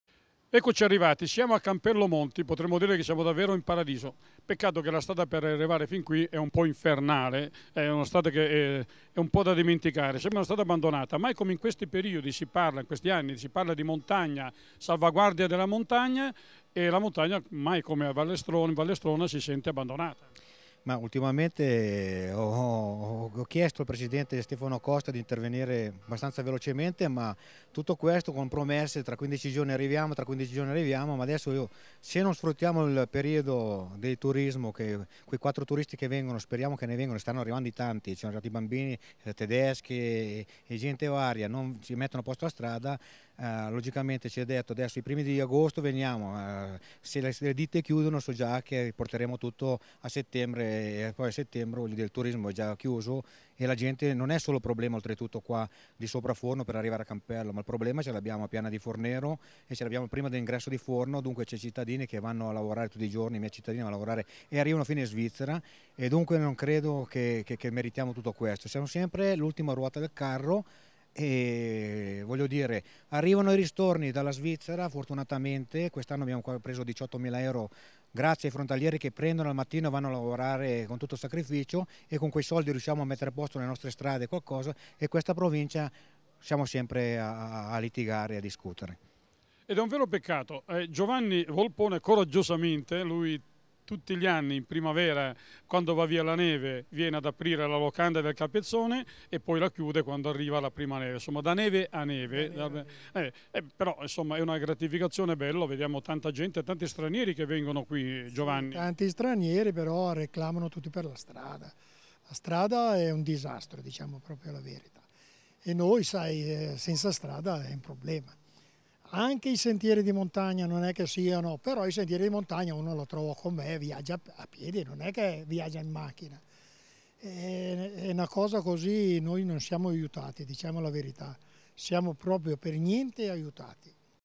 Massi lungo la provinciale della Valle Strona
31 luglio 2017 - Sassi lungo la provinciale che collega Canova Loreglia. Il sindaco Capotosti: “Siamo abbandonati”